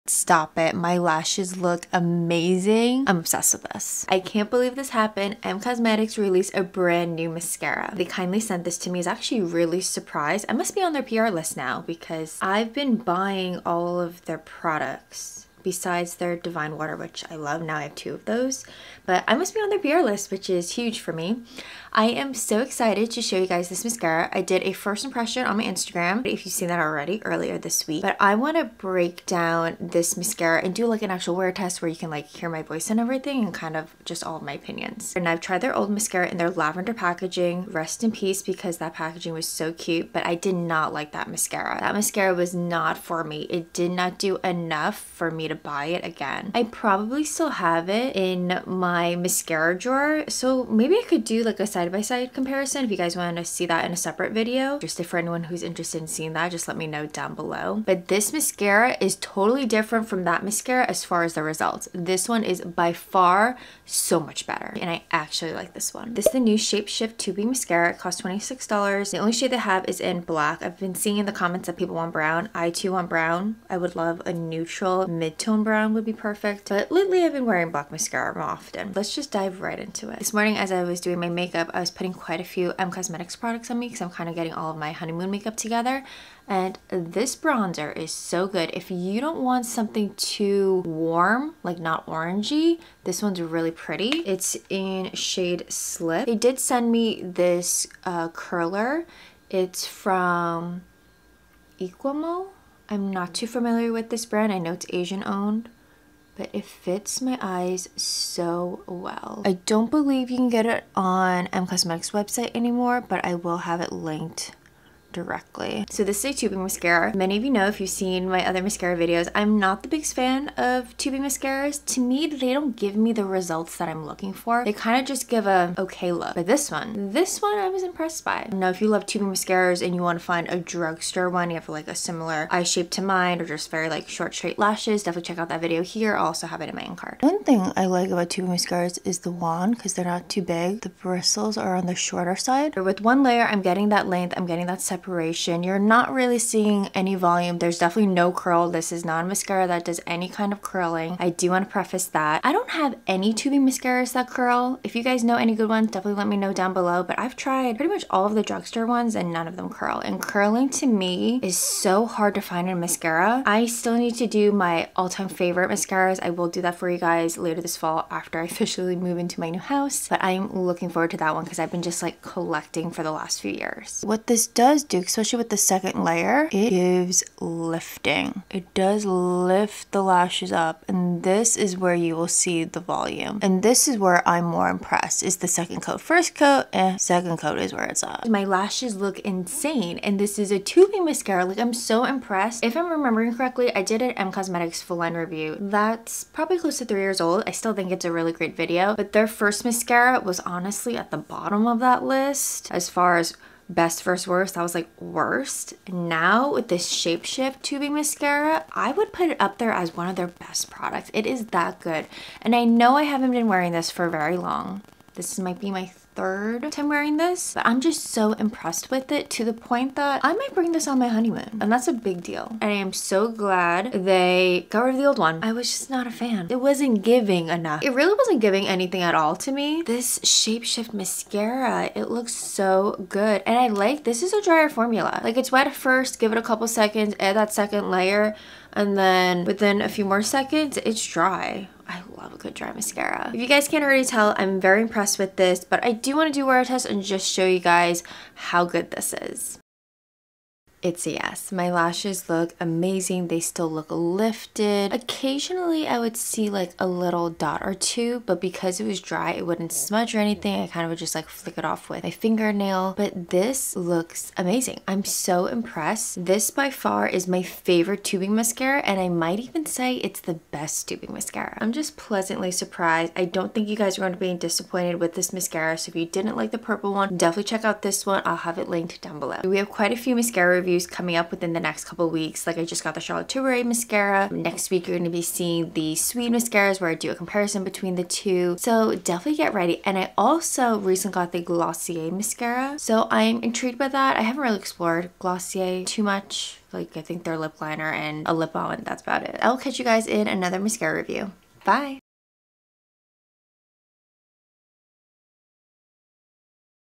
The BEST Tubing Mascara?! | Em Cosmetics Shapeshift Tubing Mascara Review (Wear Test)
This might just be the best tubing mascara I have ever tried! Today, we are testing out the new Em Cosmetics Shapeshifter Tubing Mascara.

My Favorite Amazon Products